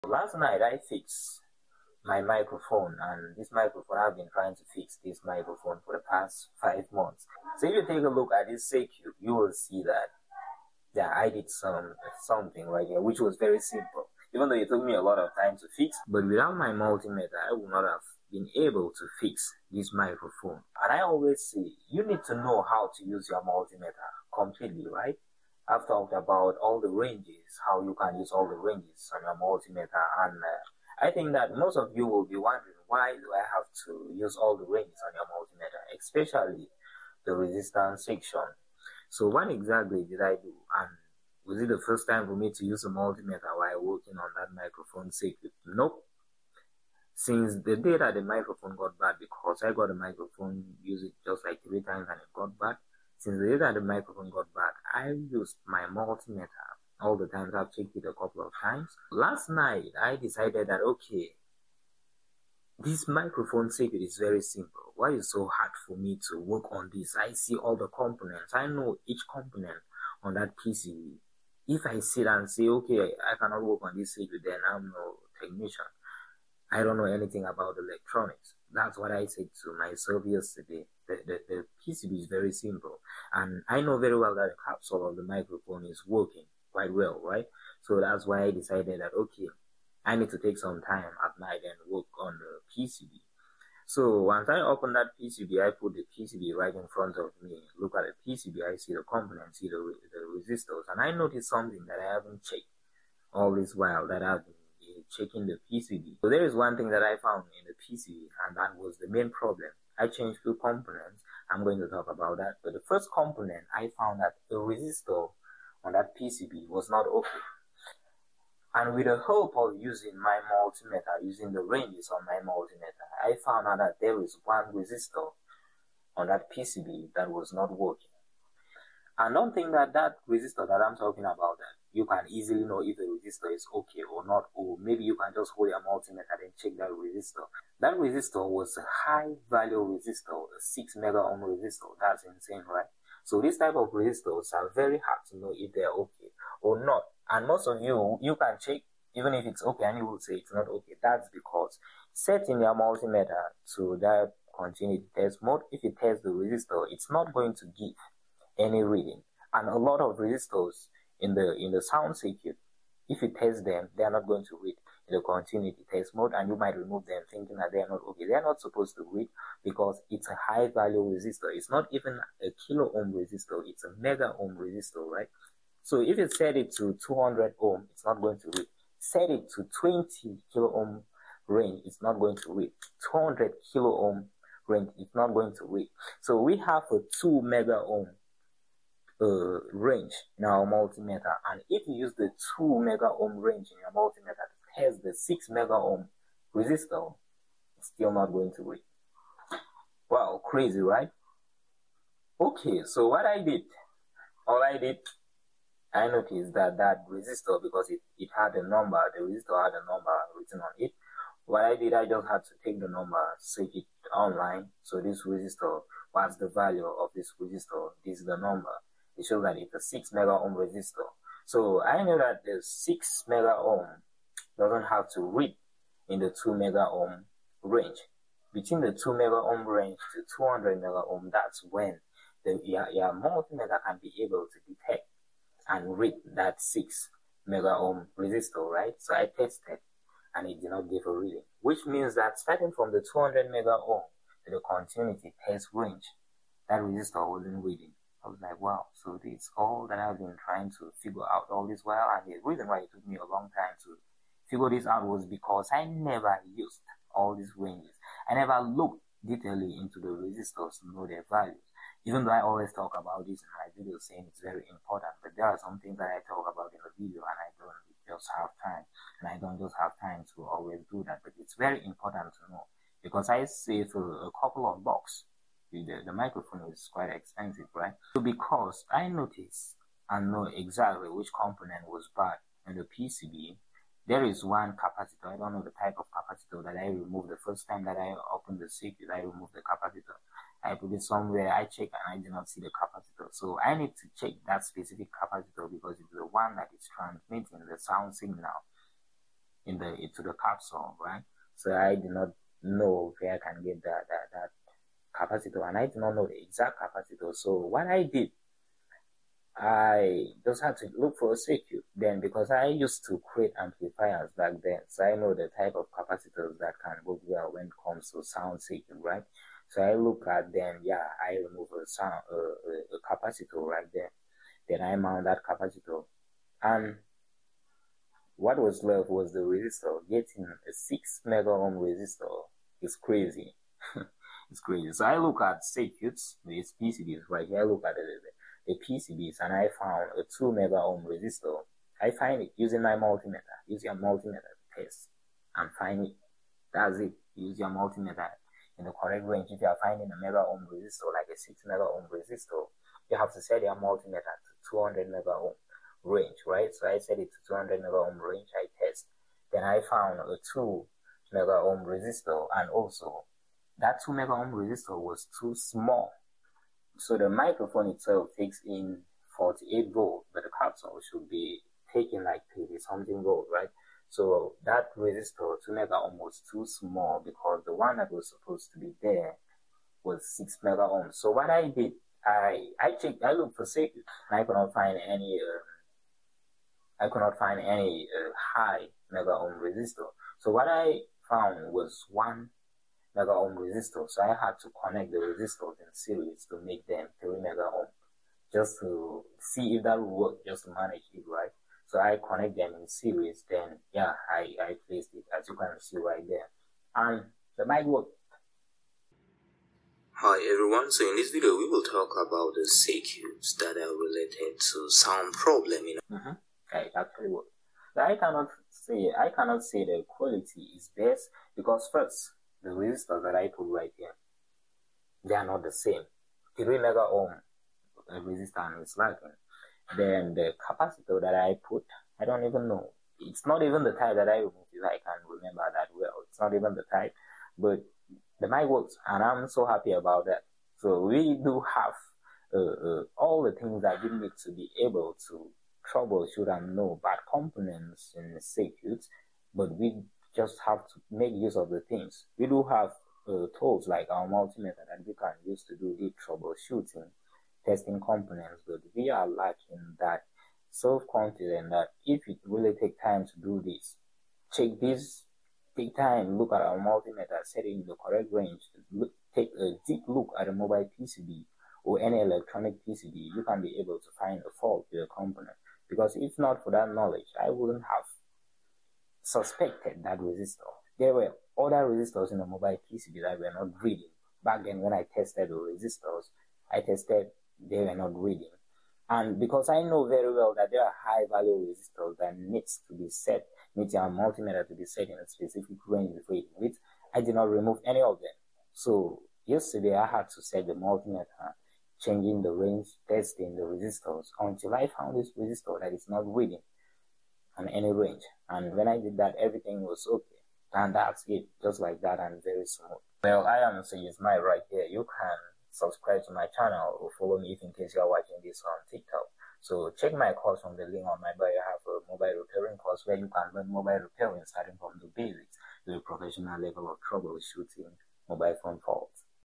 How I Fixed My Broken Condenser Microphone After 5 Months!
After 5 months of testing, troubleshooting, and learning — I finally fixed this broken condenser microphone!
In this video, I’m sharing the full journey:

✅ How I struggled with the fault for months
✅ What mistakes I made during the repair process
✅ How my multimeter became my most important tool
✅ What finally worked to fix the problem

🎯 This is not a step-by-step tutorial, but my real experience that can inspire anyone learning electronics repairing, gadget fixing, or DIY electronics projects!

If you love electronics, circuits, and real repair experiences, this video is for you.